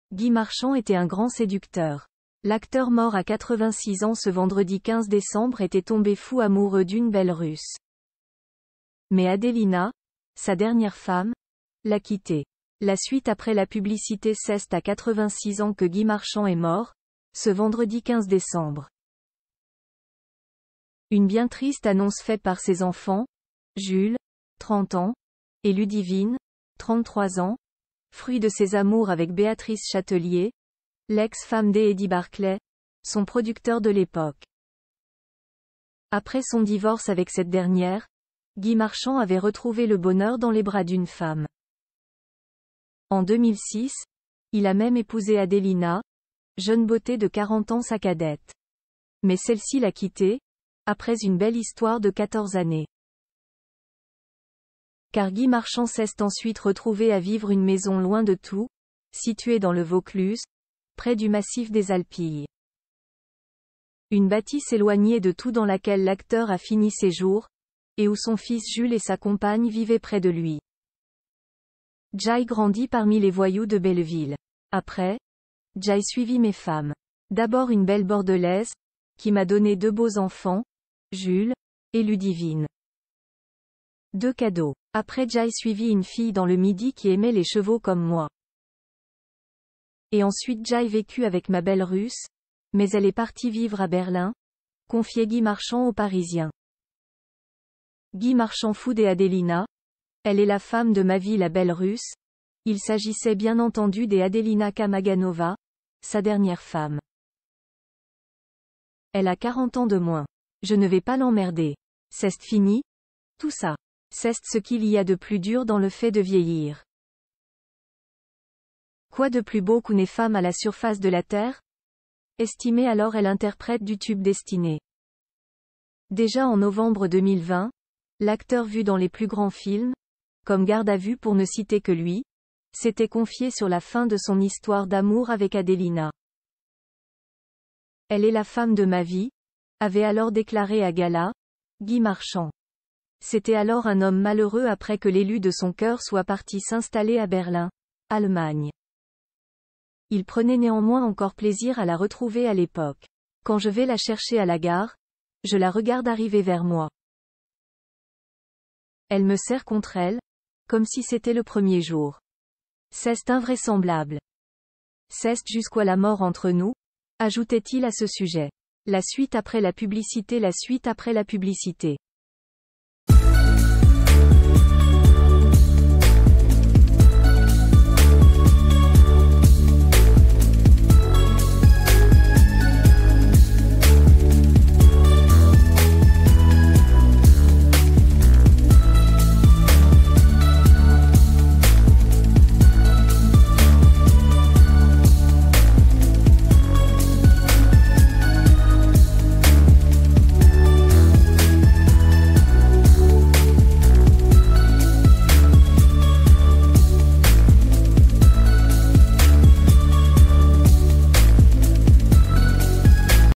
Guy Marchand quitté par sa dernière femme, de 40 ans de moins, une "belle Russe"
Guy Marchand était un grand séducteur. L'acteur mort à 86 ans ce vendredi 15 décembre était tombé fou amoureux d'une belle Russe. Mais Adelina, sa dernière femme, l'a quitté.  La suite après la publicité     C'est à 86 ans que Guy Marchand est mort, ce vendredi 15 décembre. Une bien triste annonce faite par ses enfants, Jules (30 ans) et Ludivine (33 ans) –, fruits de ses amours avec Béatrice Chatelier, l'ex-femme d'Eddie Barclay (son producteur de l'époque). Après son divorce avec cette derniè...